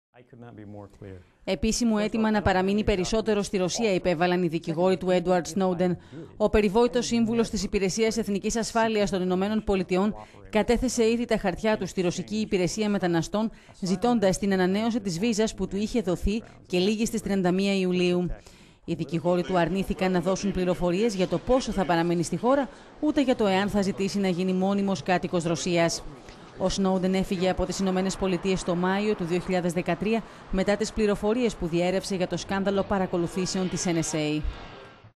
Την παράταση της παραμονής του στην Ρωσία ζητάει ο 'Εντουαρντ Σνόουντεν
Επίσημο αίτημα να παραμείνει περισσότερο στην Ρωσία, υπέβαλαν οι δικηγόροι του Εντουαρντ Σνόουντεν.

Ο περιβόητος σύμβουλος της Υπηρεσίας Εθνικής Ασφάλειας των ΗΠΑ, κατέθεσε ήδη τα χαρτιά του στη ρωσική υπηρεσία μεταναστών, ζητώντας την ανανέωση της βίζας που του έχει δοθεί και λήγει στις 31 Ιουλίου.

ΟΙ δικηγόροι του αρνήθηκαν να δώσουν πληροφορίες, για το πόσο θα παραμείνει στη χώρα, ούτε για το αν θα ζητήσει να γίνει μόνιμος κάτοικος Ρωσίας.

Ο Σνόουντεν έφυγε απο τις ΗΠΑ τον Μάιο του 2...